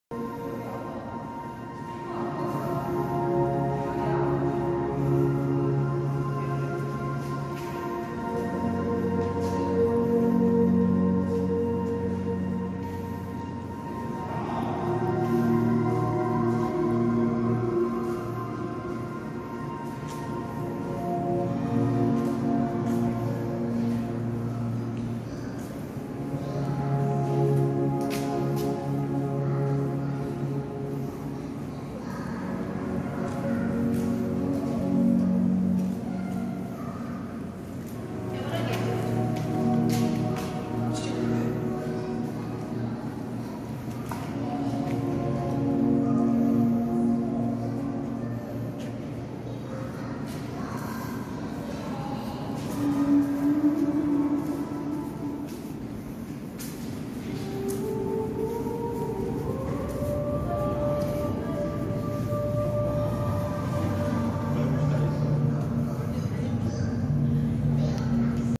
화암동굴 멀티미디어아트
2023년 01월 09일 강원도 정선군 화암면 화암리 화암동굴 멀티미디어아트 영상입니다